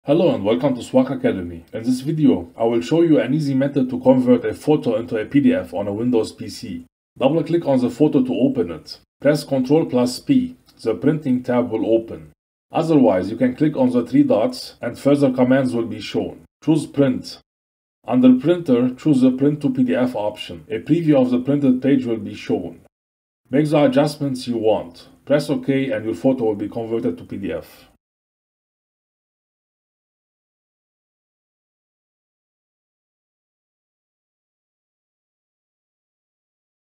How to Convert JPG to PDF on PC
In this step-by-step tutorial, learn how to convert a JPG image to an Adobe PDF on a Windows 10 PC